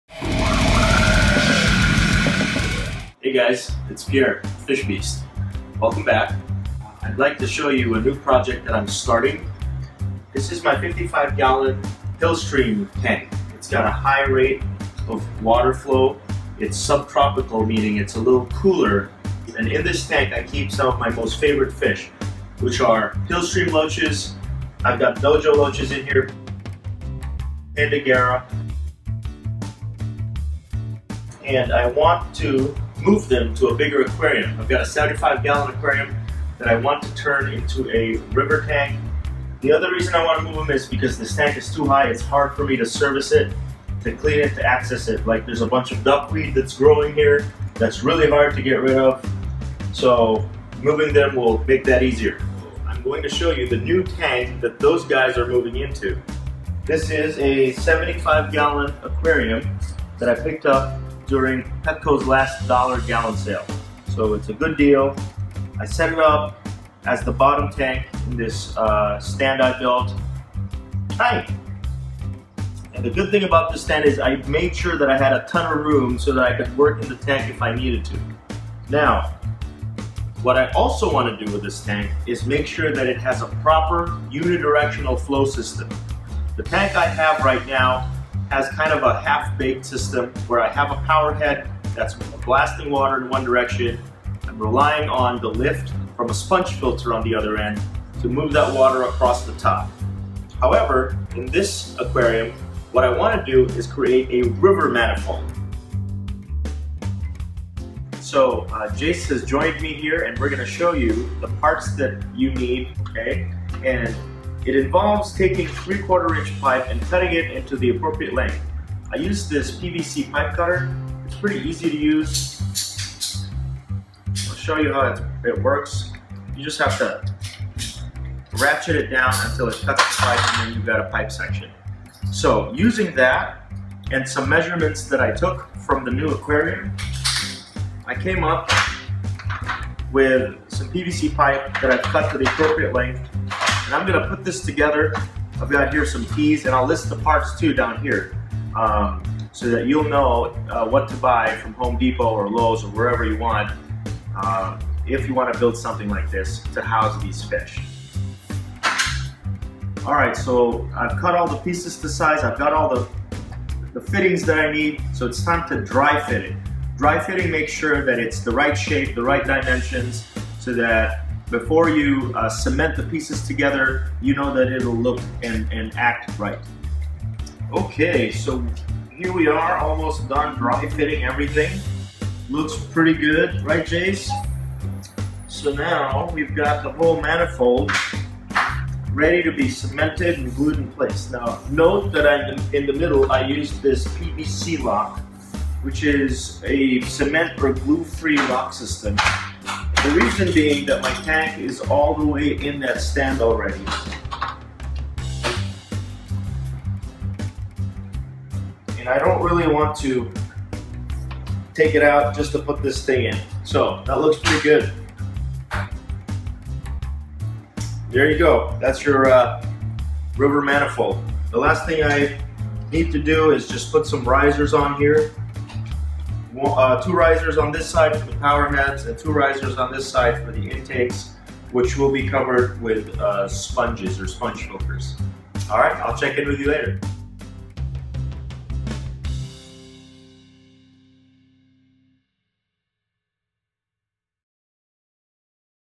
Hillstream Tank Build - Part 1: River Manifold
Hello!

This is Part 1 of my 75 gallon hillstream tank build. In this video I explain the rationale behind this project and show you the first steps of building a river manifold using PVC pipe to achieve unidirectional water flow at a high rate in a 75 gallon tank.

I am currently setting up the tank as a hillstream (fast flowing) style biotope to simulate the natural conditions in which these fish are found in the wild. Although it is possible to keep hillstream loaches in community aquariums at tropical temperatures, they seem to be more active in a fast flow environment with cooler water, in which they have evolved specific adaptations to live.

The manifold will be used for circulation and also filtration of this aquarium.

Plumbing parts:
- 3/4" PVC pipe
- (4) slip x slip elbows
- (6) slip x slip tees
- PVC cement

Other parts:
- (2) AquaClear 50 Powerheads (270 GPH each)
- (2) Aquarium Solutions Bacto-Surge Sponge Filter XL
- (2) furniture rubber feet